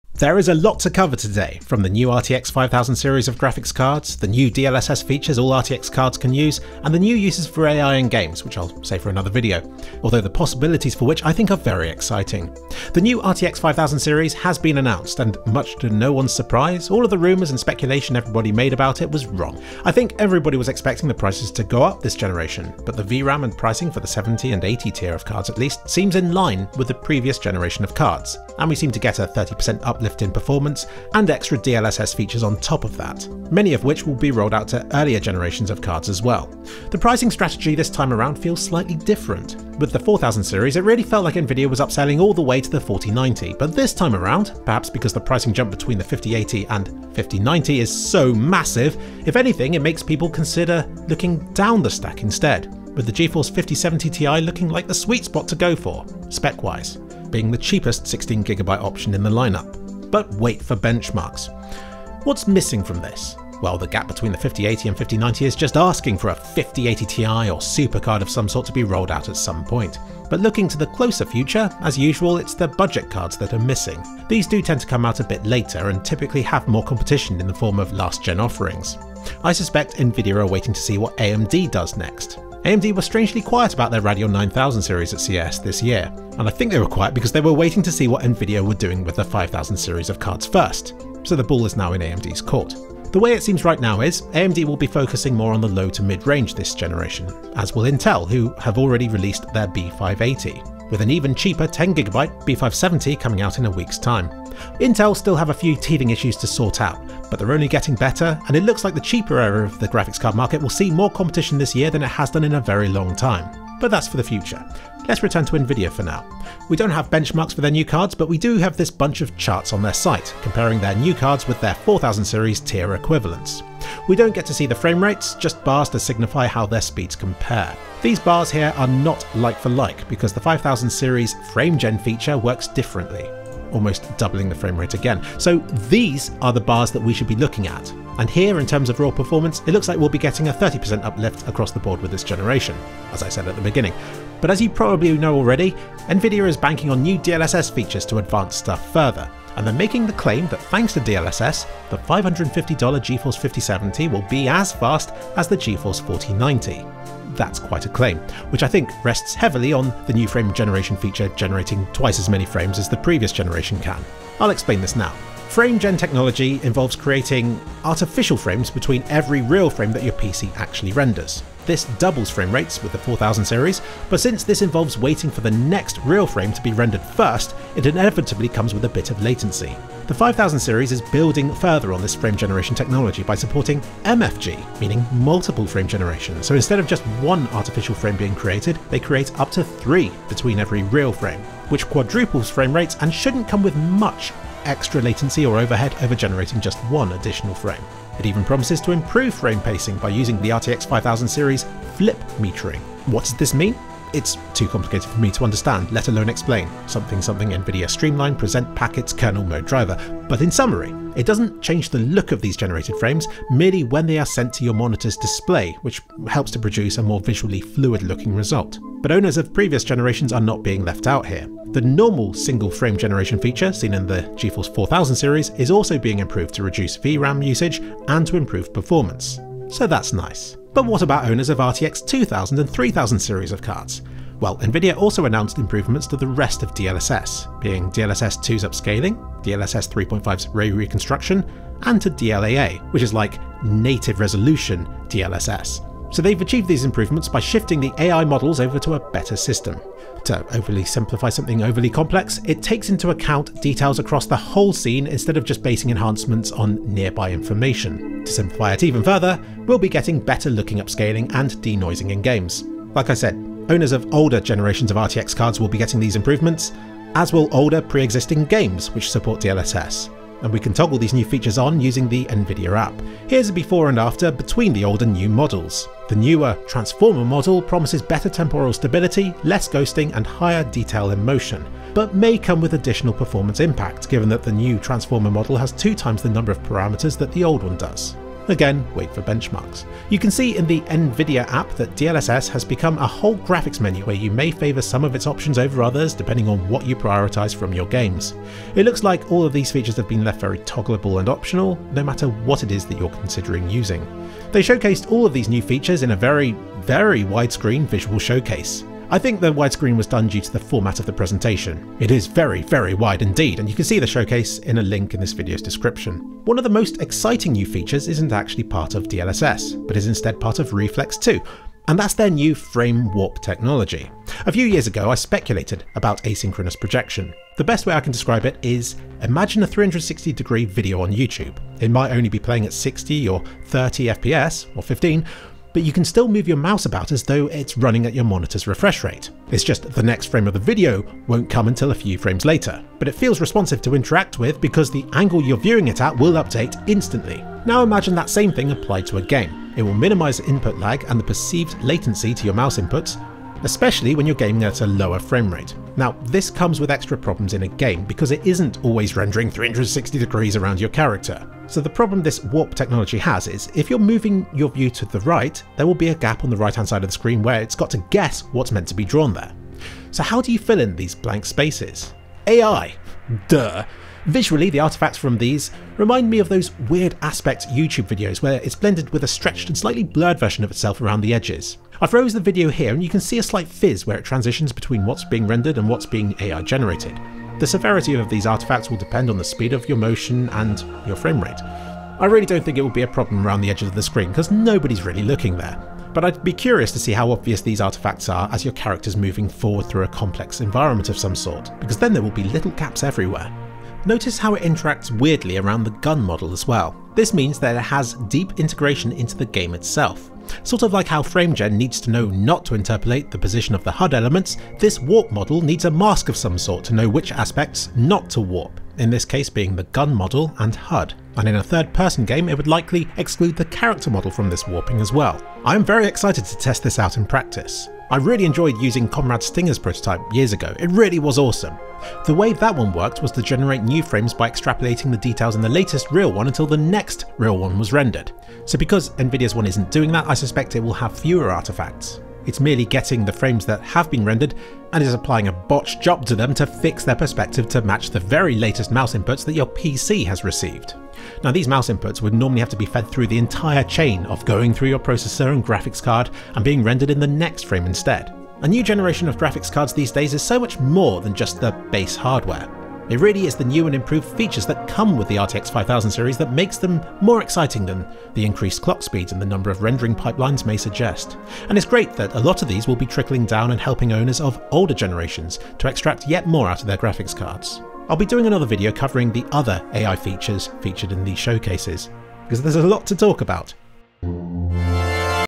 Geforce RTX 5000 Series - New DLSS and Reflex Features
The RTX 5000 Series has been announced, but DLSS also gets some new features, a few upgrades and Reflex 2 becomes half way towards my dream of what it could be.

0:00 - Pricing and GPU situation
1:55 - Speed of new cards?
2:45 - Multiple Frame Gen
3:39 - Older generations getting updates too
5:04 - Very wide visual showcase
5:16 - Reflex 2 Warp
7:38 - Hardware VS Software